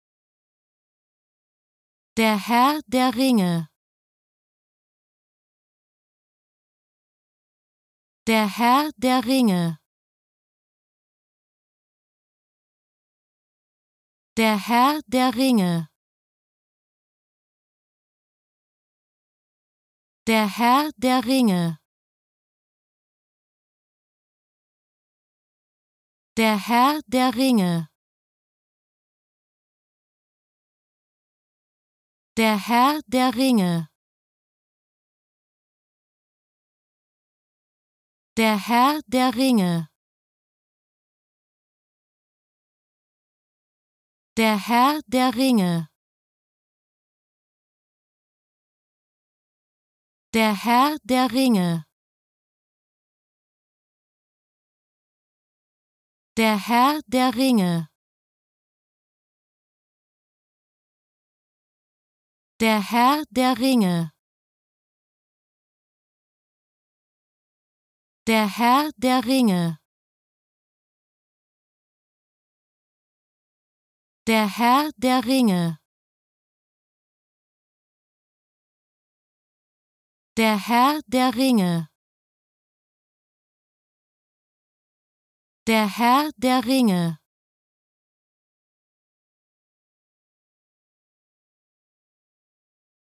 How to Pronounce ''Der Herr Der Ringe'' (The Lord of the Rings) Correctly in German
Learn how to say ''Der Herr Der Ringe'' in German with this short pronunciation tutorial.